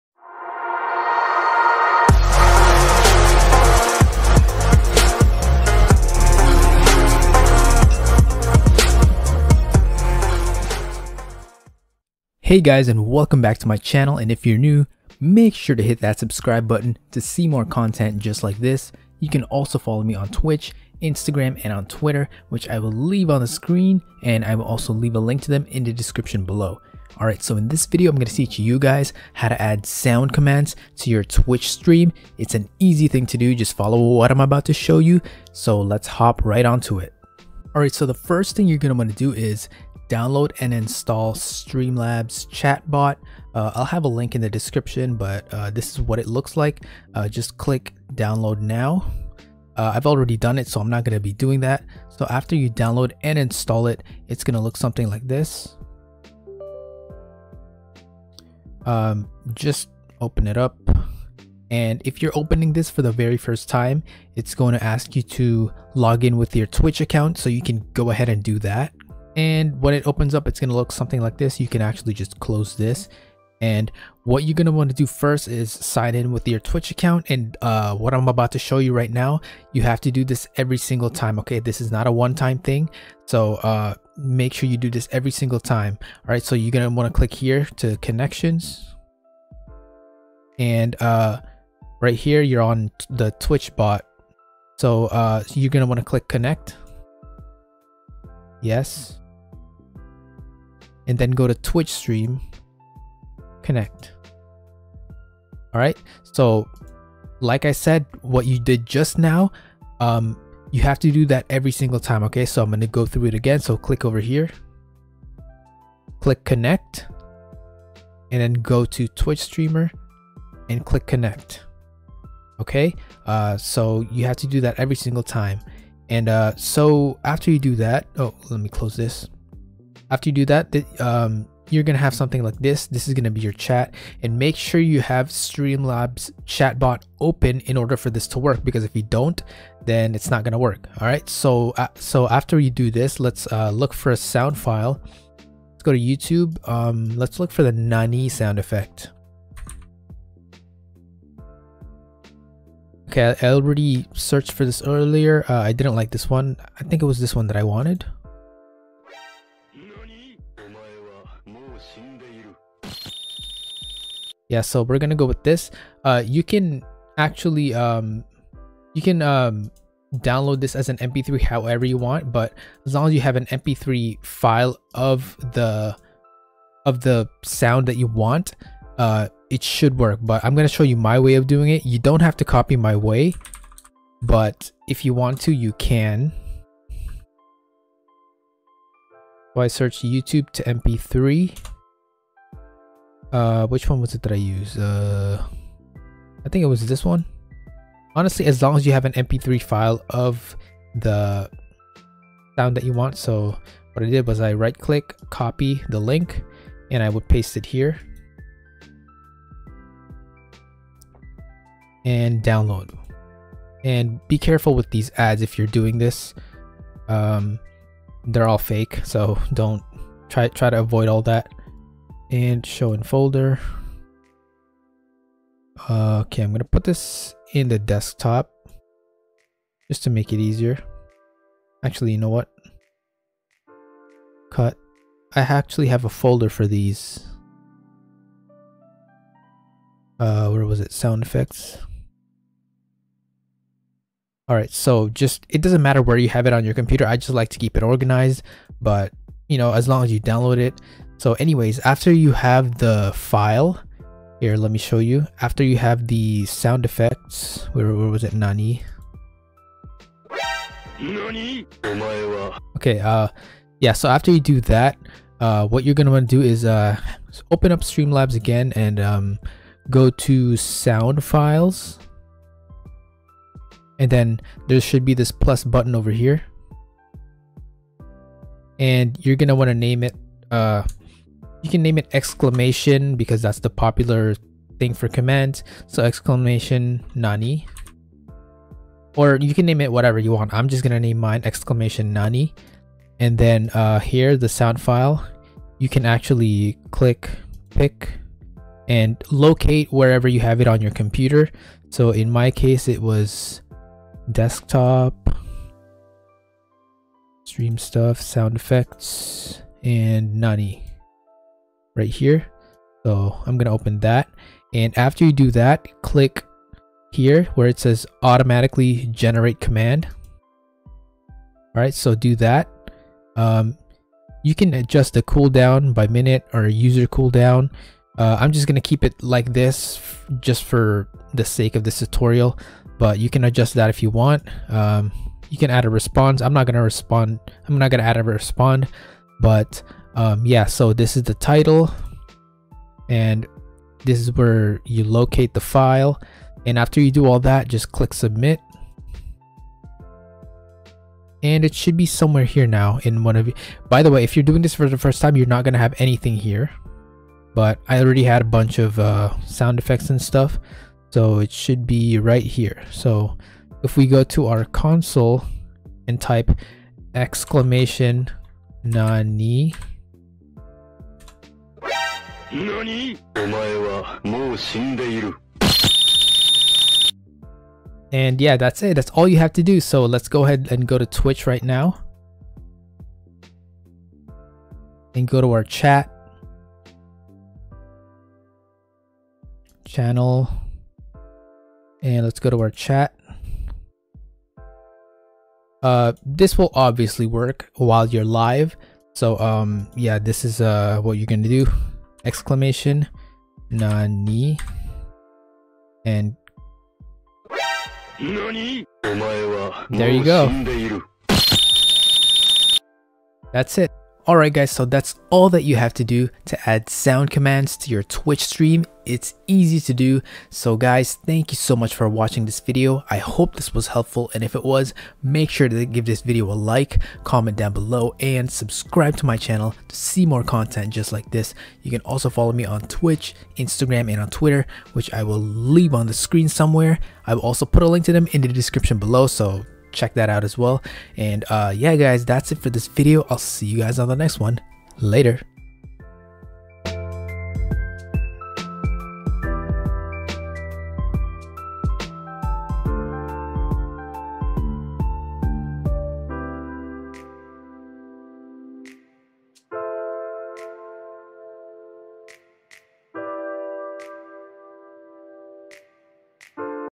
How to add sound commands to your twitch stream!
So in this video, I teach you how to add sound commands to your twitch stream using streamlabs chatbot. It's a pretty easy thing to do. This way, your twitch viewers can have some fun and troll you while you stream! I hope this video is helpful!